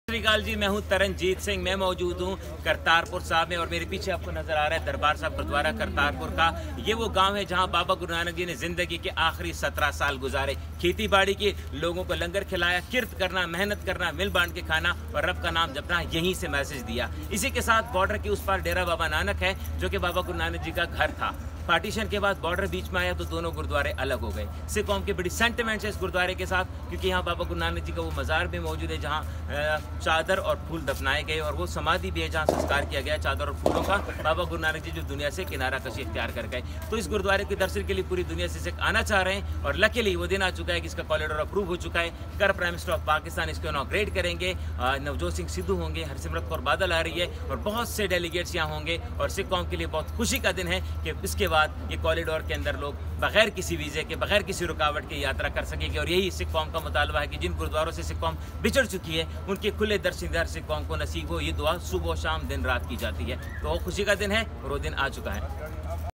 Kartarpur corridor opening ceremony by PM Imran khan
Kartarpur corridor